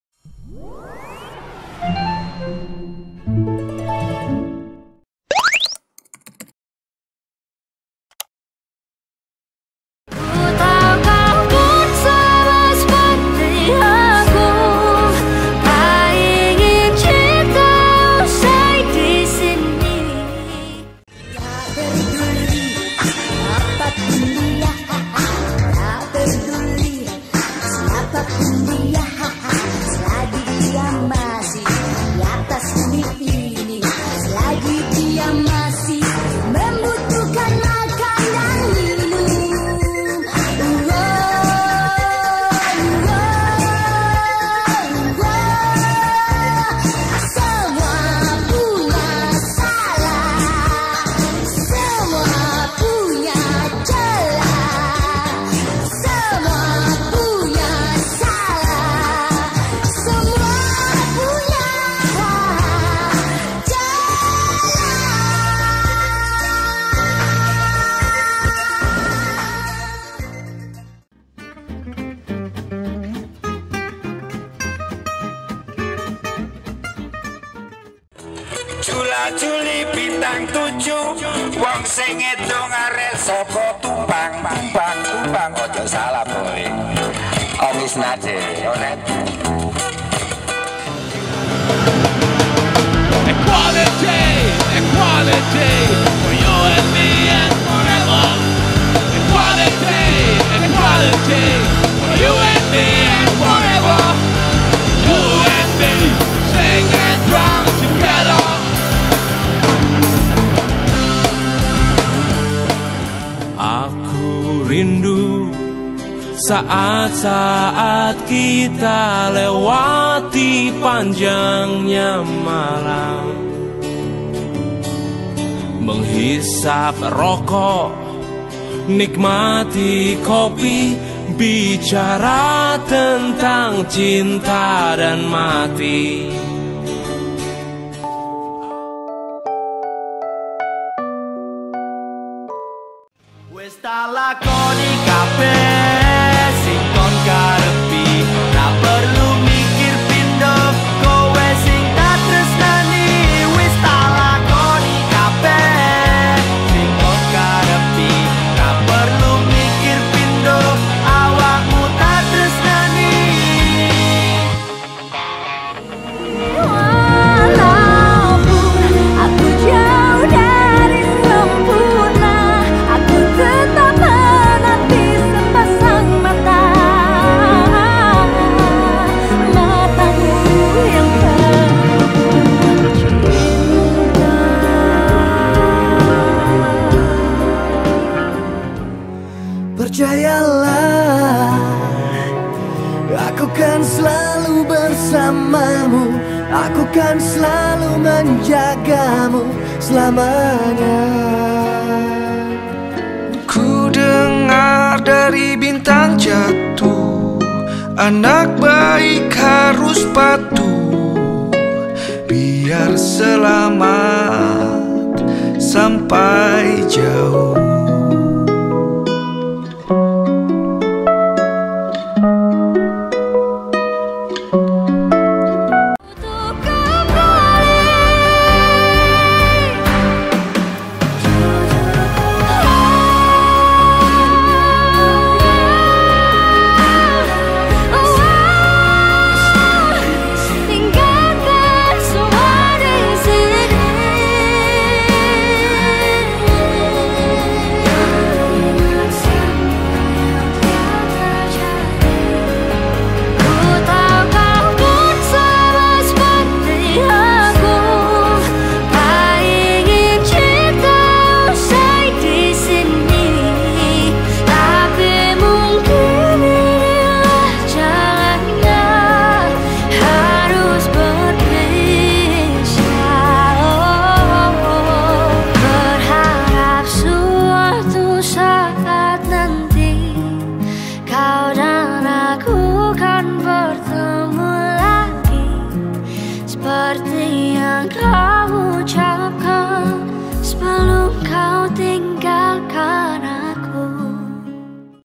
Group 7 Music Website Project Profile Video
Politeknik Negeri Malang
Kelas 2I Teknik Informatika
Jurusan Teknologi Informasi
Project Lecture Assignment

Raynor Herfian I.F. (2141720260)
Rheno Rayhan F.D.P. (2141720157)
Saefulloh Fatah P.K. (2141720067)

► PLEASE READ! ◄